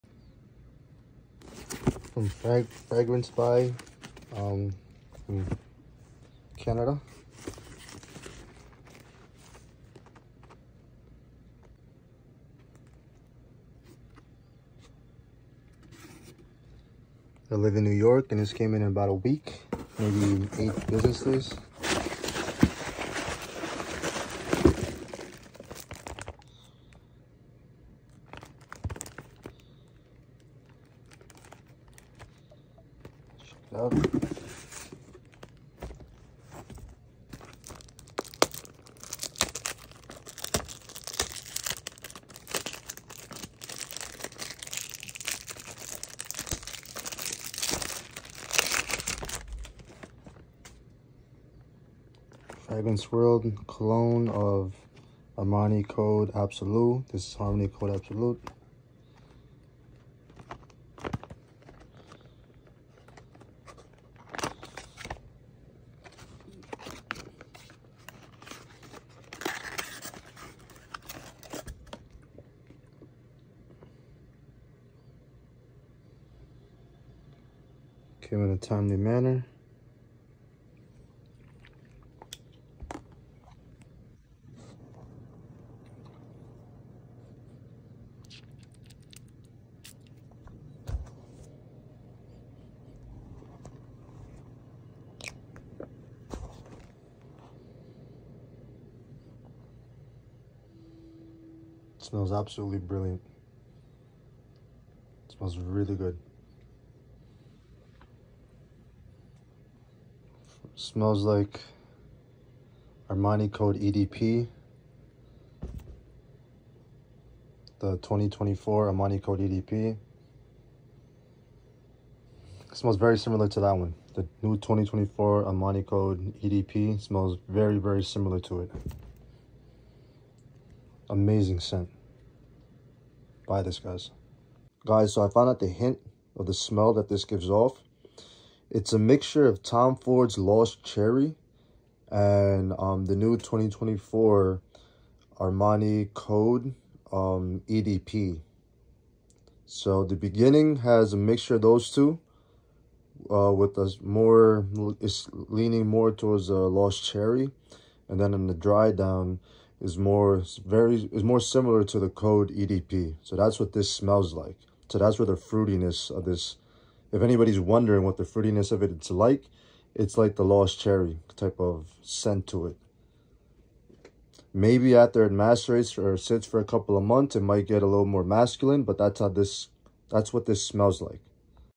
Harmony code absolute review. Fragrancebuy.ca is legit!!
Bought from fragrancebuy.ca and everything came to New York in 1 week!